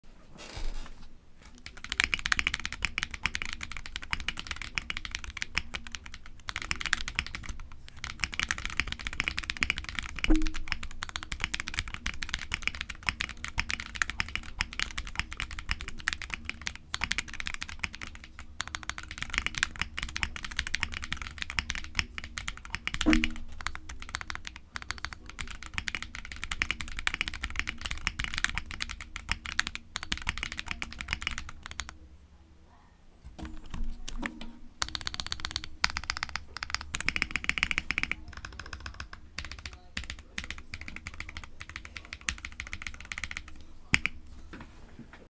QK65 | KNC Keys Green Jacket Tactile (Stock) | POM Plate sound test
Keyboard: QK65
Plate: POM full
Switch: KNC Keys Green Jacket Tactile (Stock)
Keycap: GMK Honor Light
Stable: TX (Permatex)
Foam: None
Case: Polyfill

Mic: MAONO AU-A04
Record: iPad Air 4th Generation

I might do a retest later